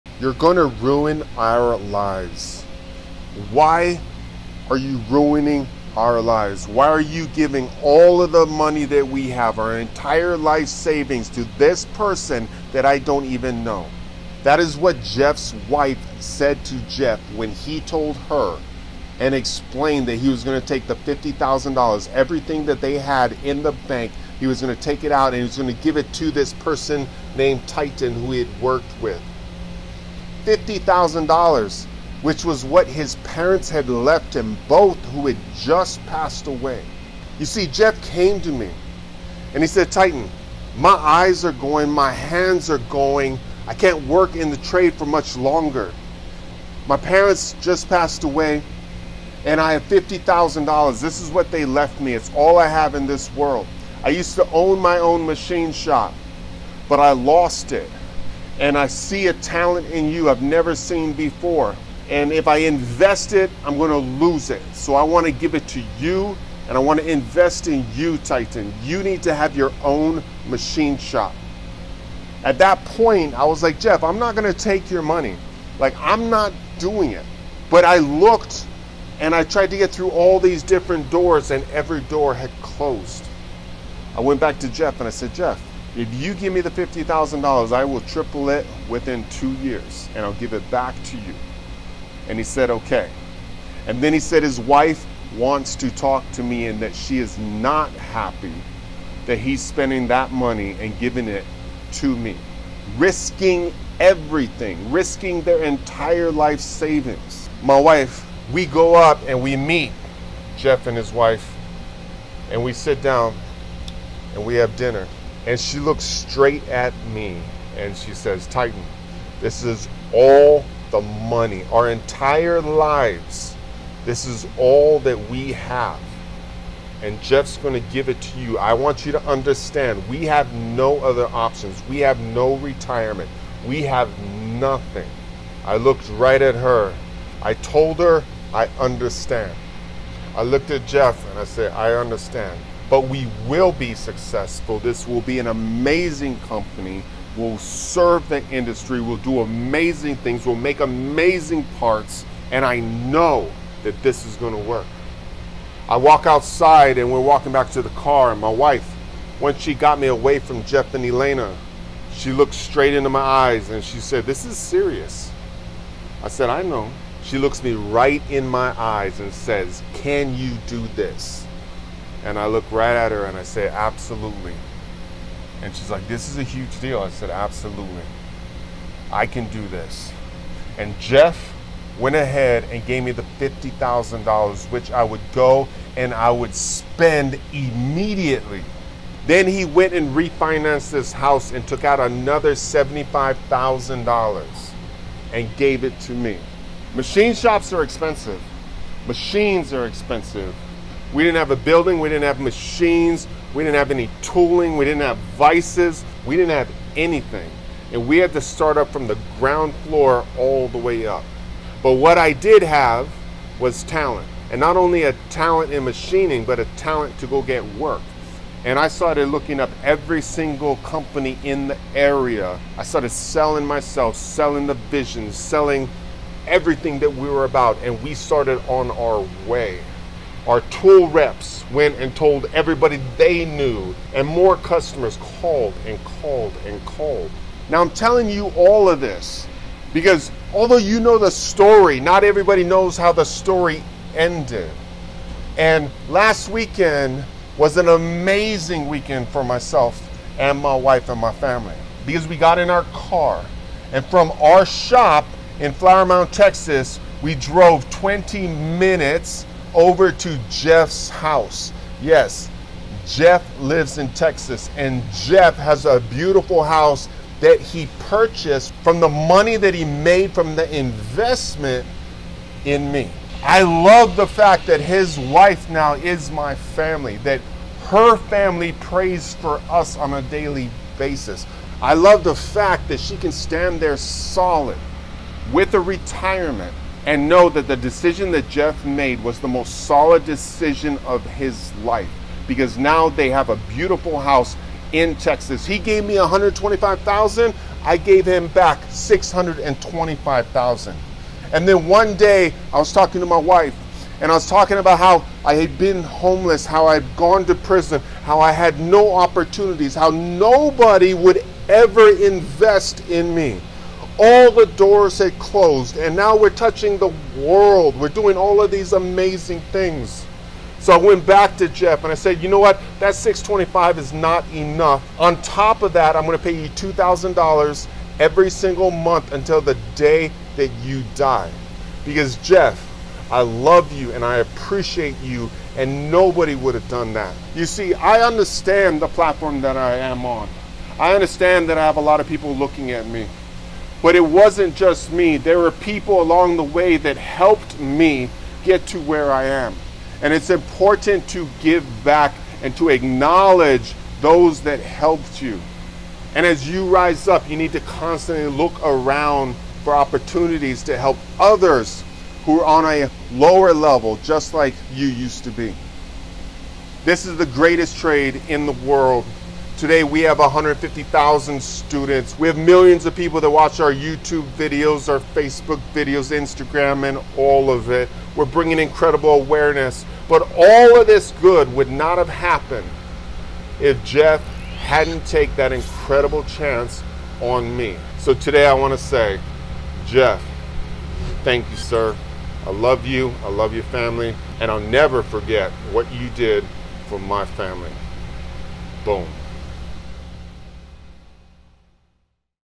My Parents Both Just Died… They Left Me $50,000… Take IT!
This is the craziest manufacturing story of all time. They invested in me, gave me all their money and I will be forever grateful.